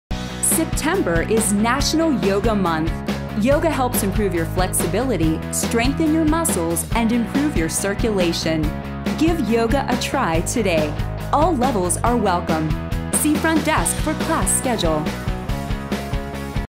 September is National Yoga Month
Yoga helps improve flexibility, strengthen muscles and improve circulation. Give yoga a try today at LA Fitness